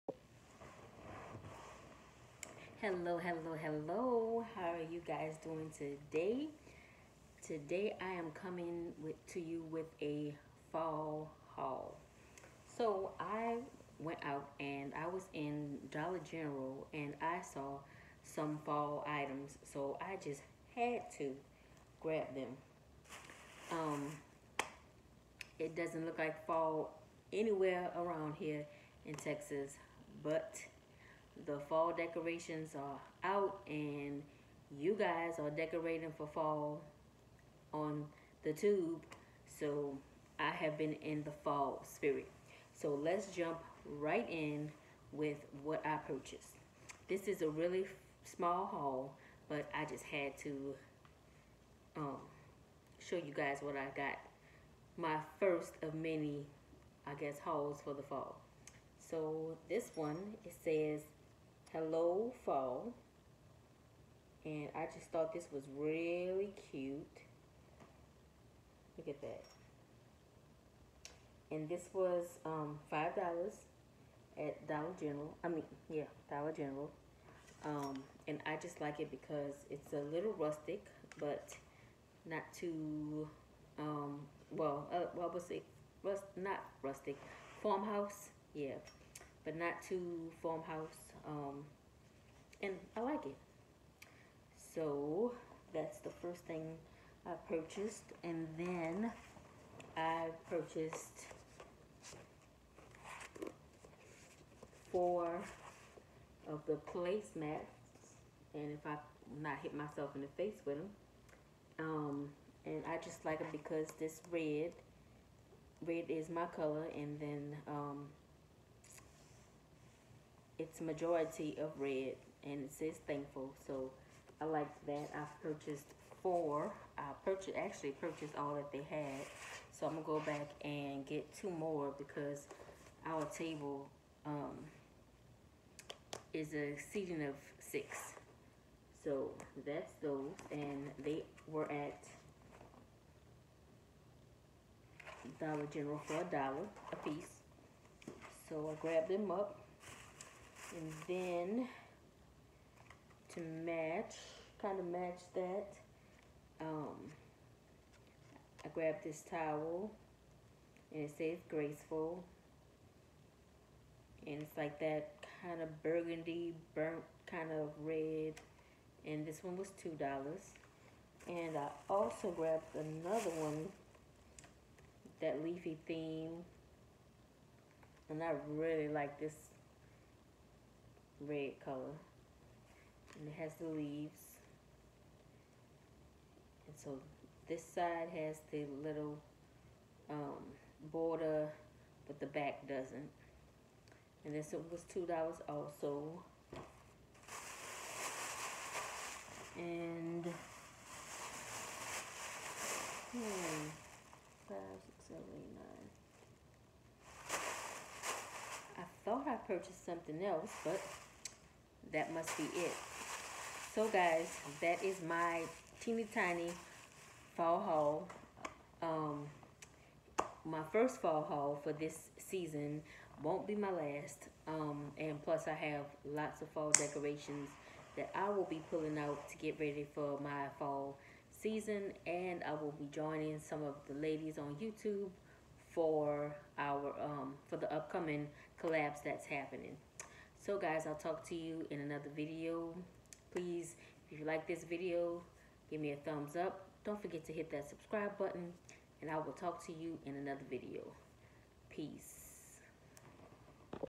Fall Shopping|My first Fall Shopping Haul of 2019| Dollar General Fall Haul
In this video I did a fall haul from Dollar General to kick off the fall season of 2019